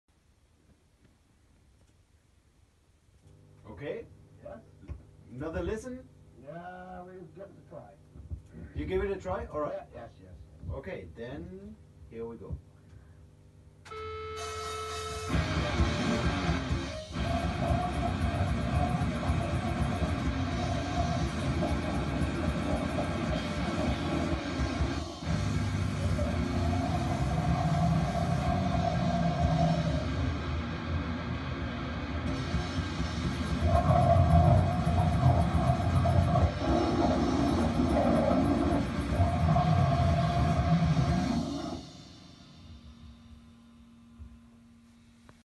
Last Days Of Humanity - 2019 studio report new song
Last days of humanity studio report.  Raw mix of a new song for the 2020 upcomming album. Recorded at Die Tonemeisterei in Oldenburg Germany on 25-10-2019